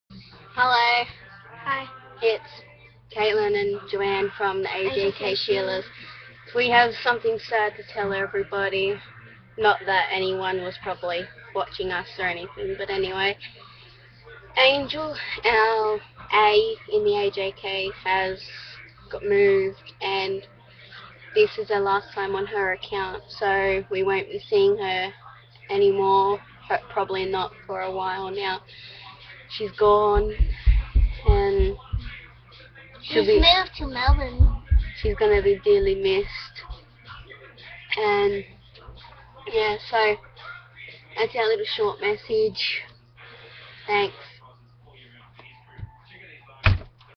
Goodbye Angel :( xox
we used angels account for the last goodbye video for her, since she could be moving.. and staying in melbourne... so anyway... AJK is no more. :(